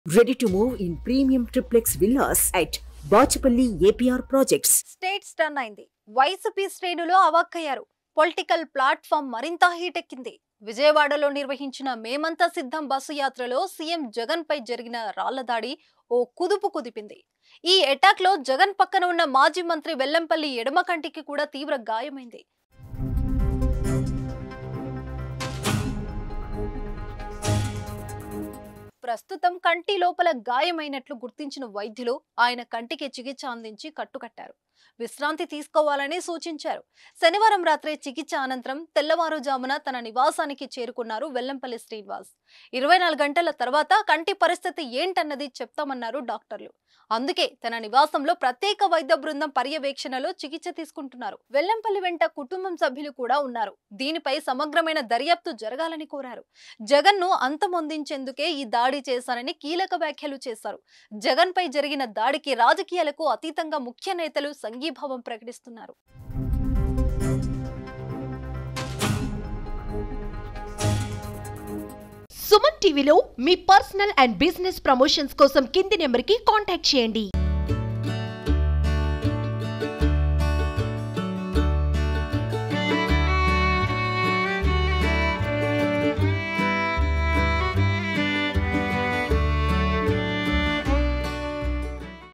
జగన్ దాడి గురించి వివరించిన వెల్లంపల్లి | Stone Attack on YS Jagan
Watch► జగన్ దాడి గురించి వివరించిన వెల్లంపల్లి |  Stone Attack on YS Jagan | @sumantvkadapa